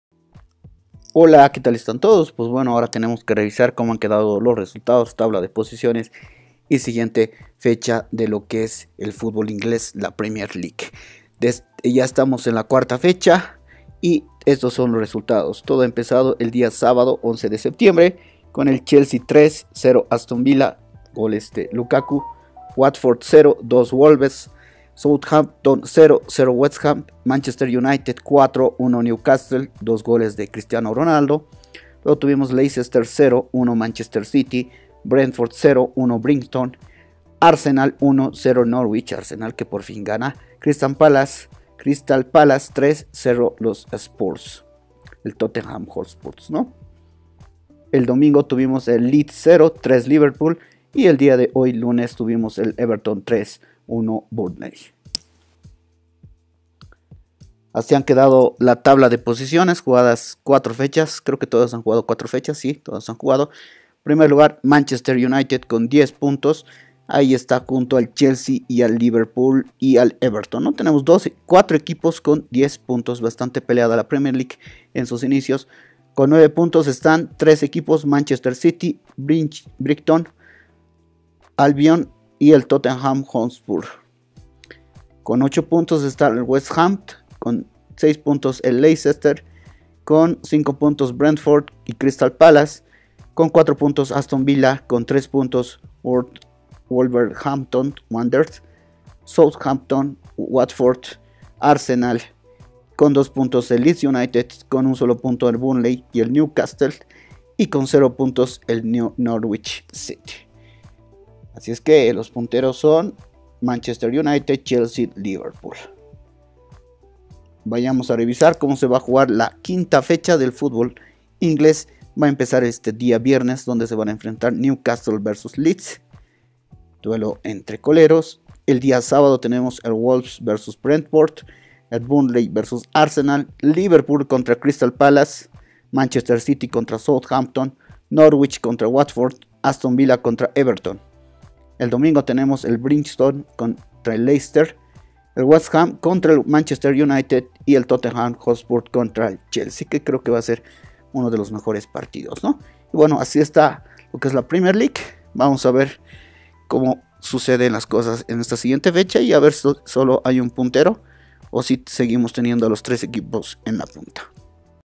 Premier League 2021 2022 , Fecha 4 , Resultados, Tabla de posiciones, siguiente fecha
Estos son los resultados de la fecha 4 de la Premier League de Torneo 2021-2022 pueden revisar